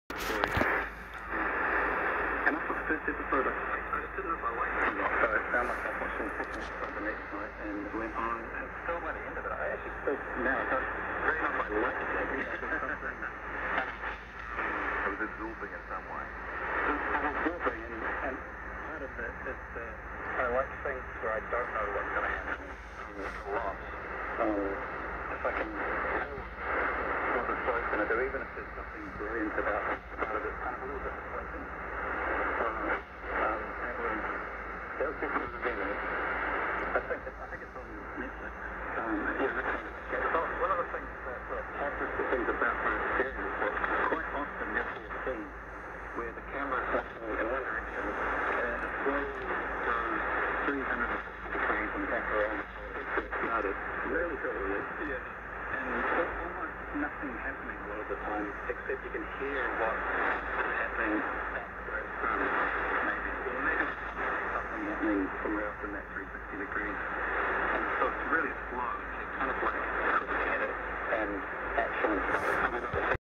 Radio New Zealand 15720 MHz
Radio New Zealand as received in Berthoud Colorado, USA at 02:45 UTC 20:45 Local